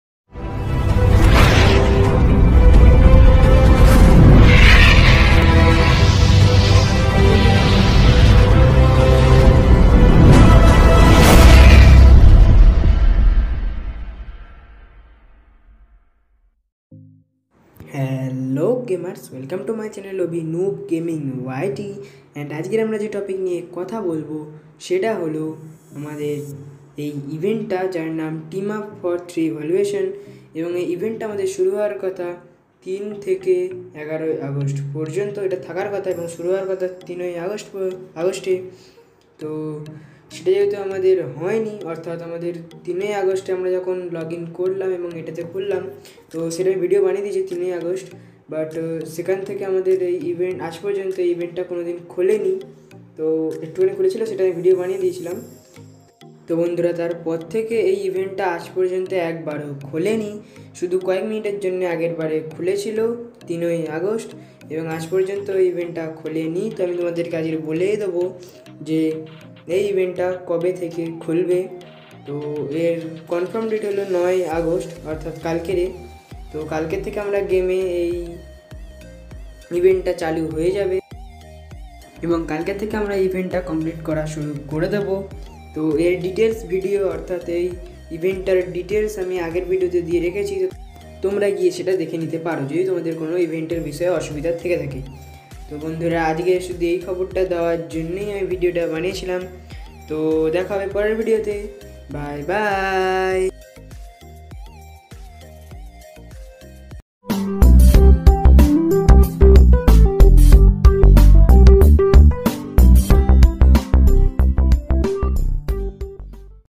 New event ta kobe open hobe. Jene nao sobar age with full details of new event.
And ajker amra je topic niye kotha bolbo seta holo eiii event ta kobe theke open hobe.
Eii event mane team up for 3volution.

Ar comment e bolte vulbe na kemon laglo tomar eiii video ta .

My Instagram name is--Abhi_roy_3049.

My Facebook name is--Abhi Roy.

Bye bye.